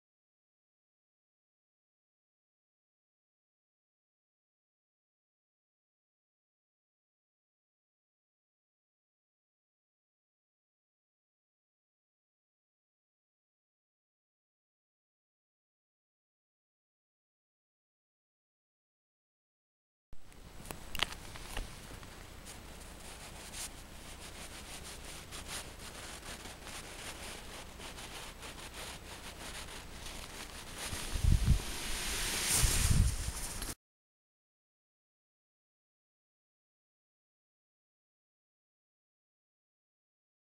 Surface Hoar Avalanche 16 Dec 2010
The YC Ski Patrol triggered this small avalanche near a study plot just out of bounds on a 22 degree slope.  It slid on a layer of surface hoar buried 17 inches deep when the third skier crossed the slope.  This slope is east facing at 9000 ft near Big Sky.